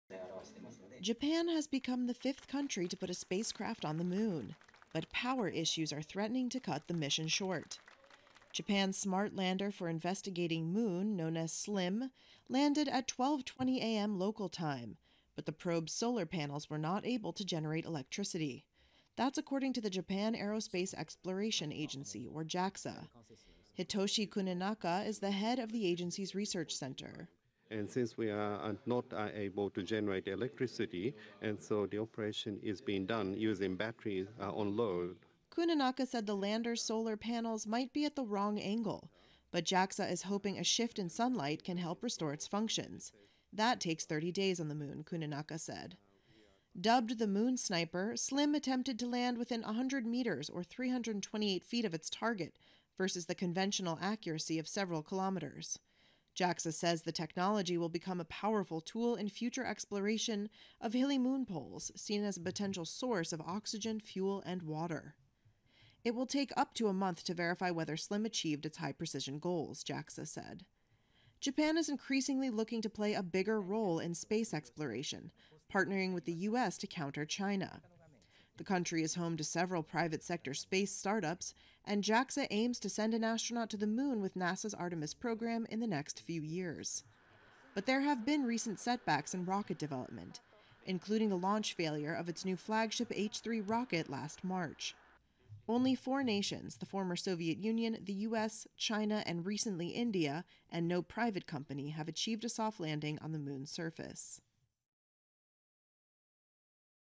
Japan’s 'moon sniper' lands successfully but struggles with power
Japan on Saturday (January 20) became the fifth country to put a spacecraft on the moon but the probe was not generating solar power, its space agency said, during a mission to prove a "precision" landing technology and revitalize a space program that has suffered setbacks.